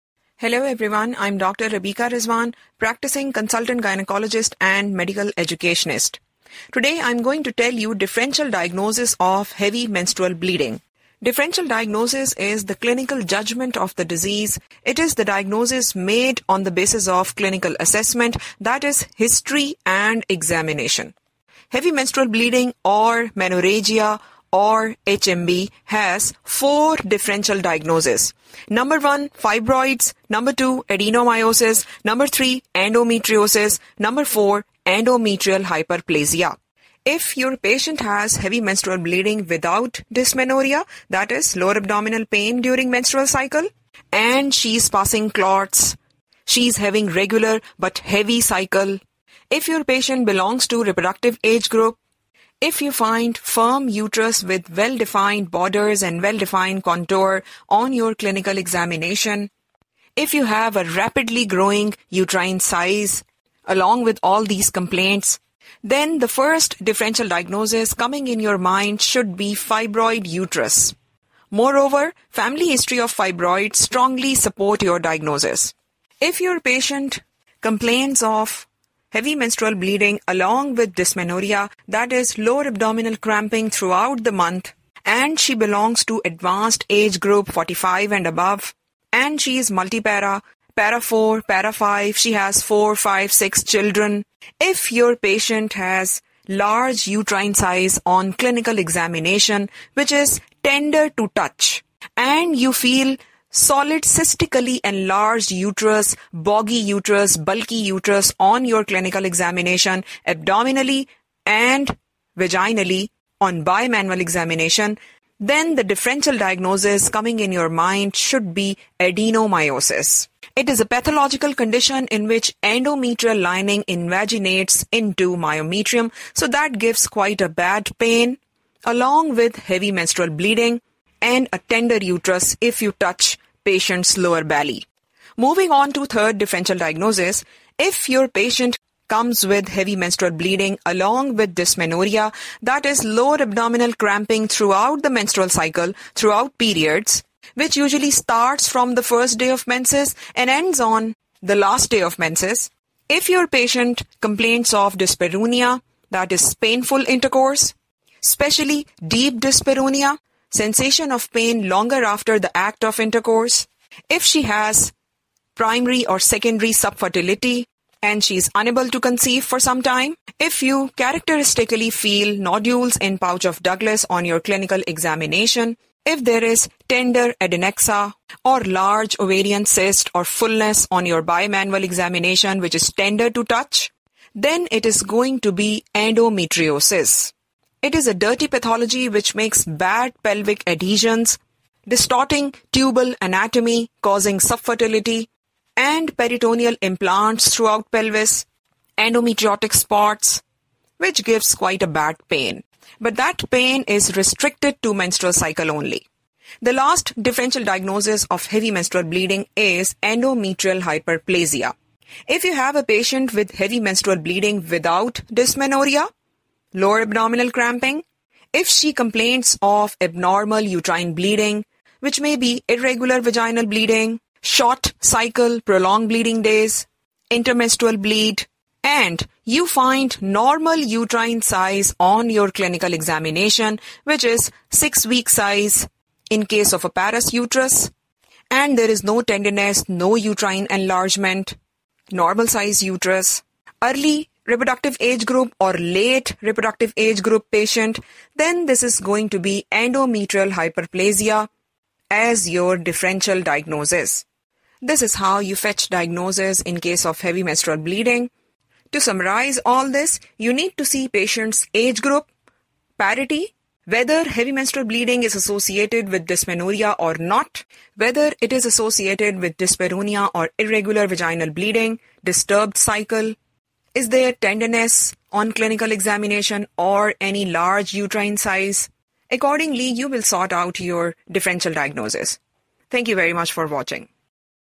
Differential Diagnosis HMB | DD Heavy menstrual bleeding by Dr.Rabika Rizwan
DD is made on clinical assessment. HMB is very commonly seen problem in society and it is important topic for exam.